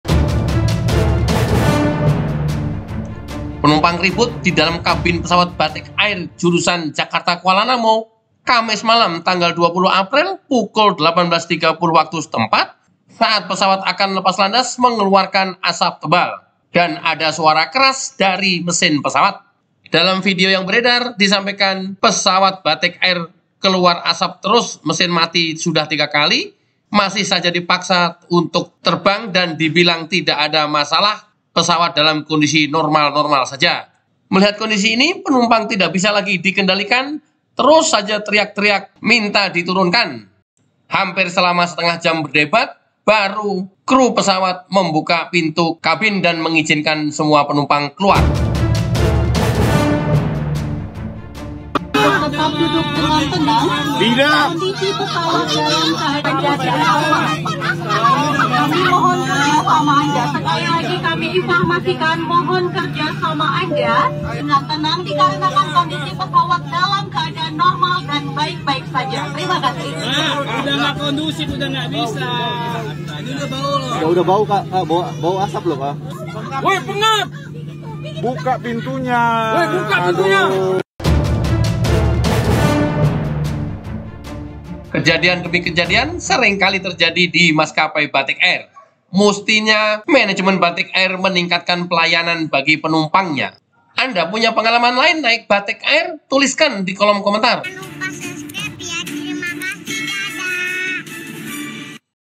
BATIK AIR DIDUGA RUSAK DAN MEMAKSA TERBANG JAKARTA - KUALANAMU, PENUMPANG RIBUT DAN TERIAK-TERIAK
Pesawat Batik air keluar asap dan mesin sempat mati 3x, tetapi masih dipaksa terbang dan crew bilang tidak terjadi apa-apa pesawat dalam kondisi baik-baik saja, penumpang terus teriak-teriak minta turun hampir 1/2 jam baru pintu pesawat dibuka dan semua penumpang turun, peristiwa terjadi Kasmis 20 April 2023 jam 18.30